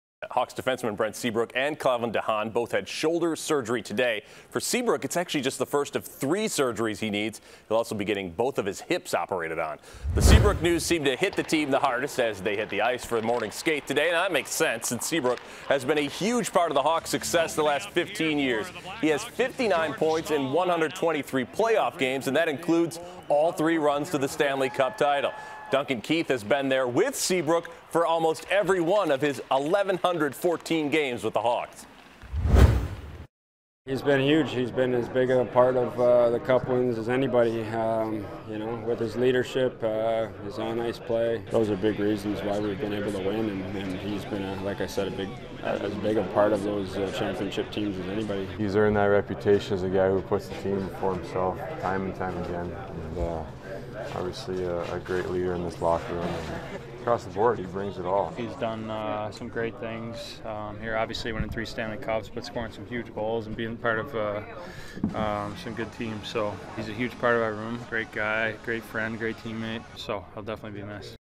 Brent Seabrook’s Absence Already Felt By Blackhawks
Chicago Blackhawks defensemen Brent Seabrook and Calvin de Haan both had shoulder successful surgery today. For Seabrook, it is actually just the first of three surgeries he needs. He will also be getting both hips operated on.